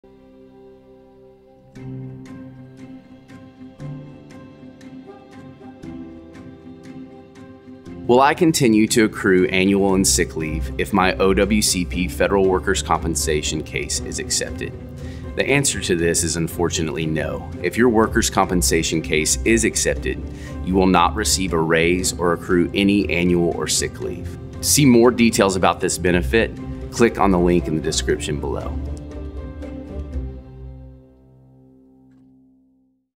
Will I continue to accrue annual and sick leave if my OWCP case is accepted?
Unfortunately, if your Workers’ Compensation case is accepted, you will not receive a raise or accrue any annual or sick leave.